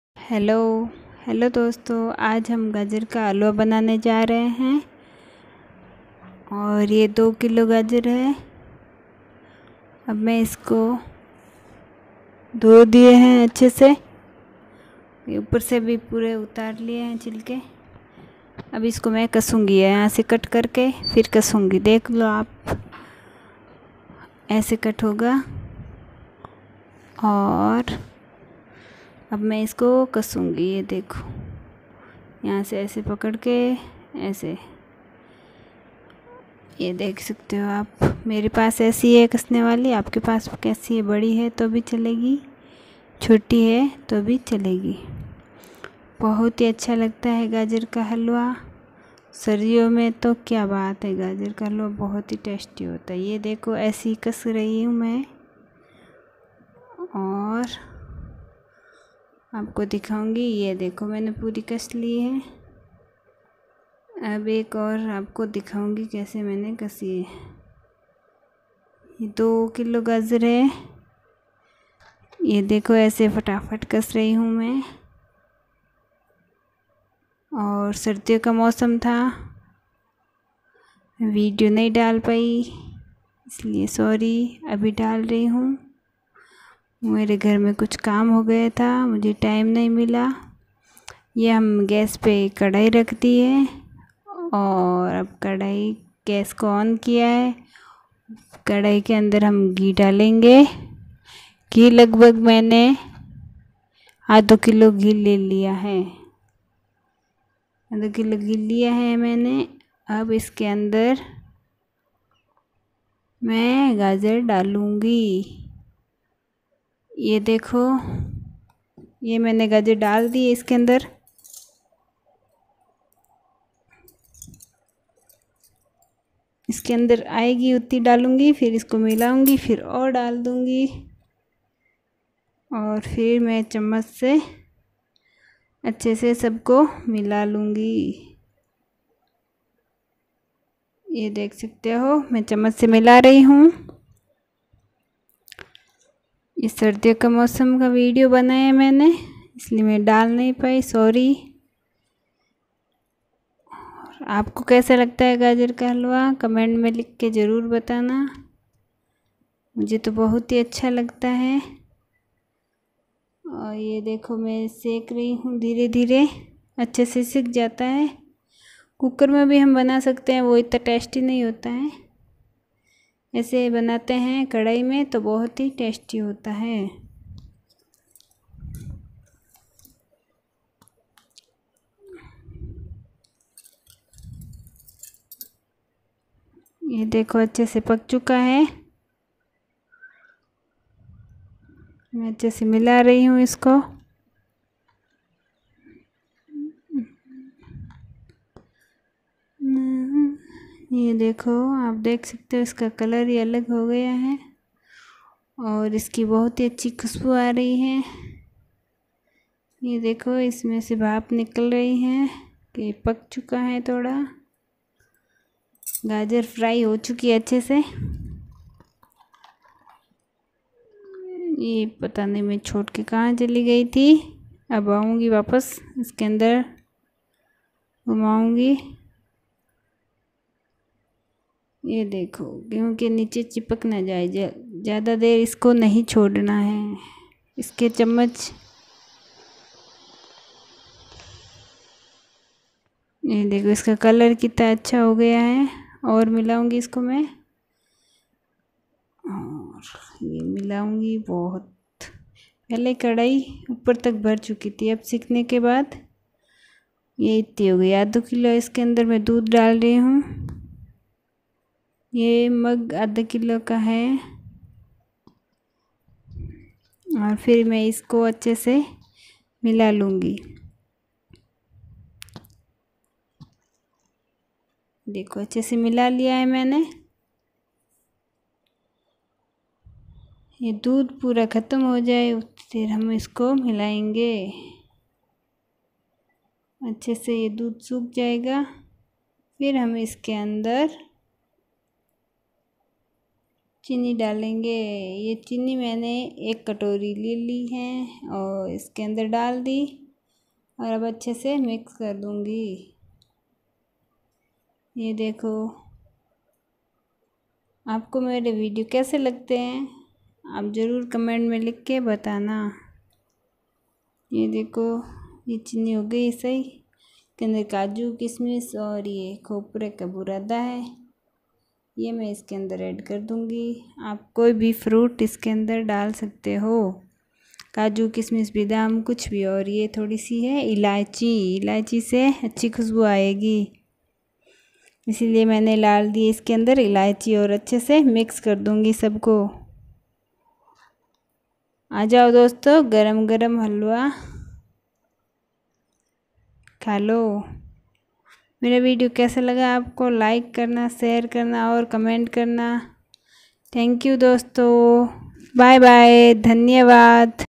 गाजर का हलवा कैसे बनाते हैं | Carrot Pudding By Housewife Sagar
आज के इस विडियो में आपको, गाजर का हलवा कैसे बनाते हैं, मैं आपको गाजर का हलवा बनाकर बताऊंगी विडियो को देखने के बाद आप घर पर स्वादिष्ट गाजर का हलवा बना सकते हैं ।